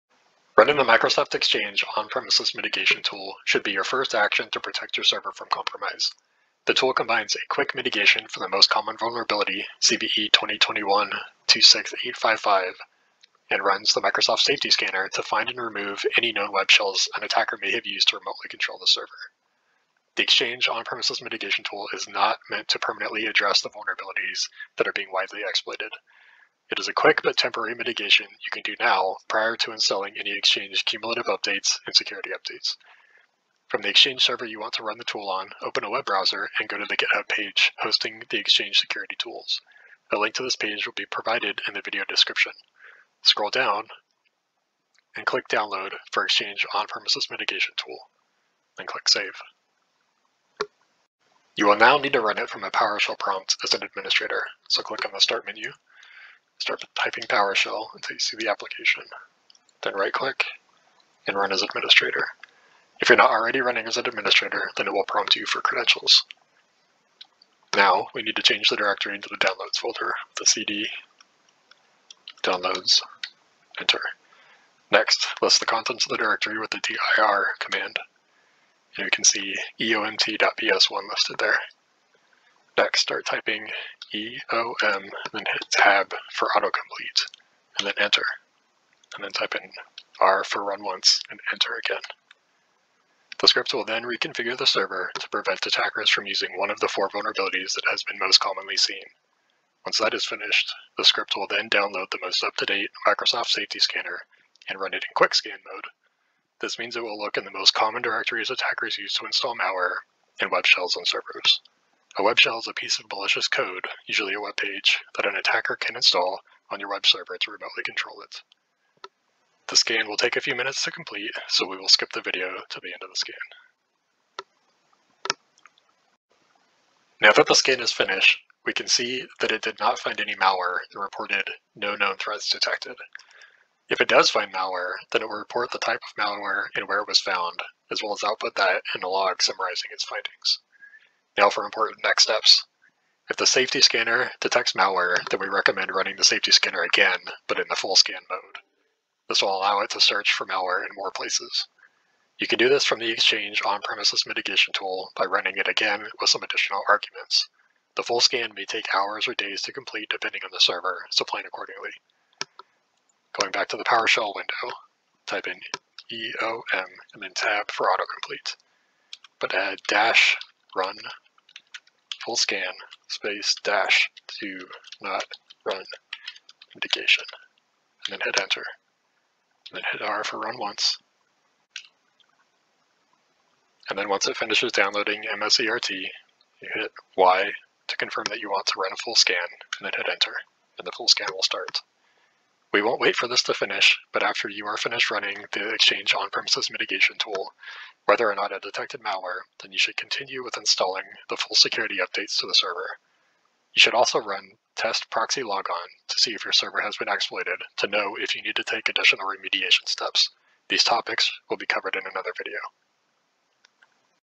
How to use Exchange On-Premises Mitigation Tool to investigate Microsoft Exchange Server threats
This guidance will help customers address threats taking advantage of the recently disclosed Microsoft Exchange Server on-premises vulnerabilities CVE-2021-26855, CVE-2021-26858, CVE-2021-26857, and CVE-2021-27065.

In this video, we demonstrate how to use Exchange On-premises Mitigation Tool a tool made publicly available by Microsoft, to mitigate the risk of compromise and help investigate whether your Exchange Server has been compromised. See Guidance for responders: Investigating and remediating on-premises Exchange Server vulnerabilities for additional details.

For security reasons, never browse and download the Exchange On-premises Mitigation Tool directly on your Microsoft Exchange Server.  Download and copy the tool to your Microsoft Exchange Server from another machine.  This tool will require outbound communication capabilities on port 443 and port 80.